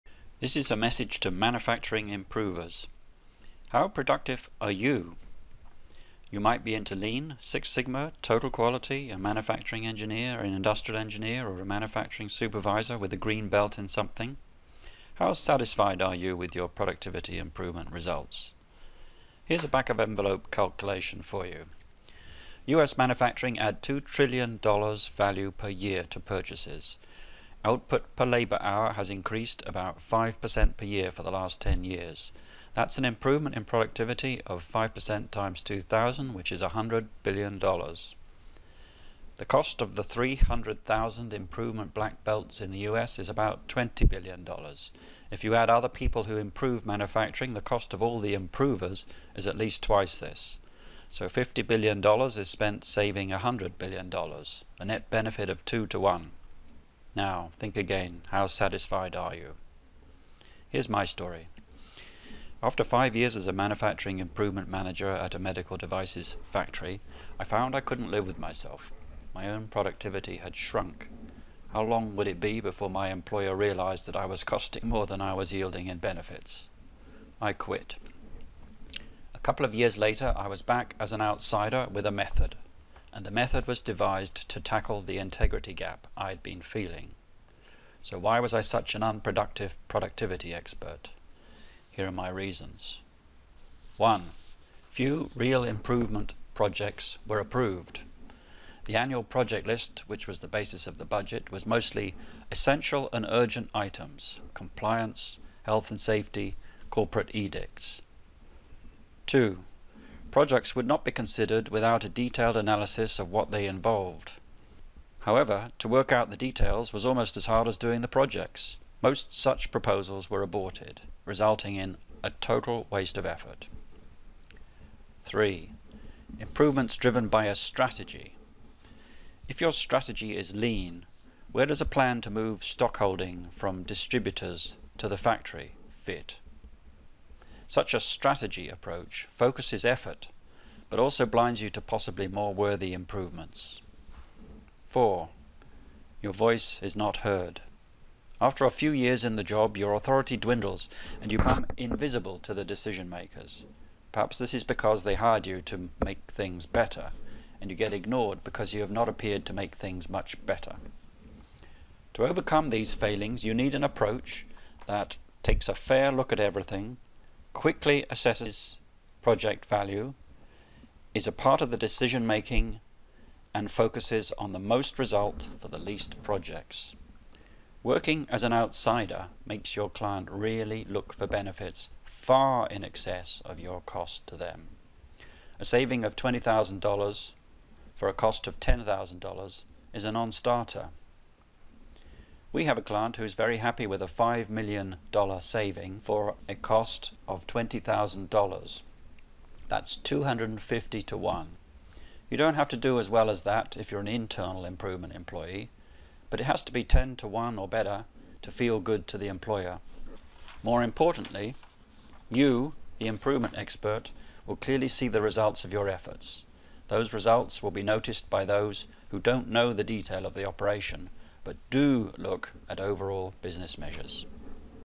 Productivity of Productivity Experts
James LaTrobe-Bateman of reMODEL International Ltd. discusses the question... How Productive are Manufacturing Productivity Improvement Experts?